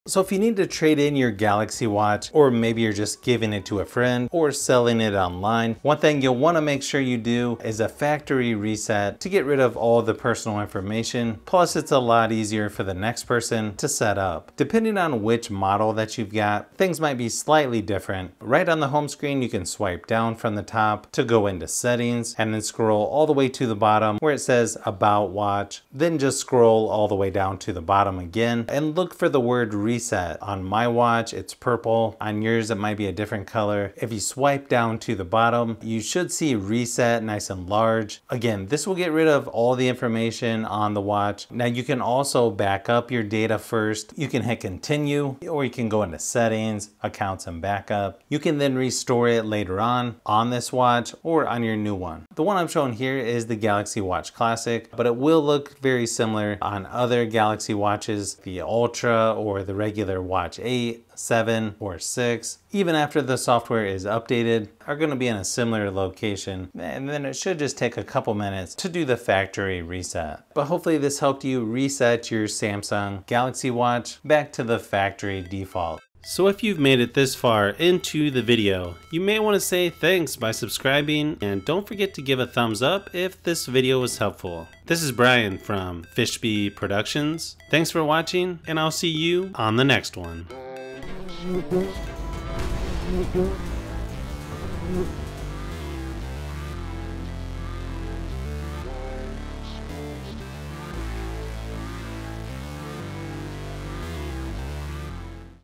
How to Reset Samsung Galaxy Watch to Factory Default
Today I'm showing you how to factory reset your Samsung Galaxy Watch before you trade, sell or give your phone to someone else.